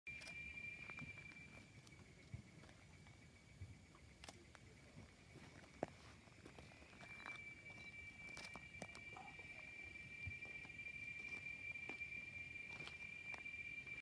ディレクタタテハEpiphile dilecta ワイルドスマコ 2018/10/28 WildSumaco Napo, Butterfly of Ecuador
A Wild Butterfly Chase　蝶をめぐる冒険
Sunstreak Tours, Butterfly of Ecuador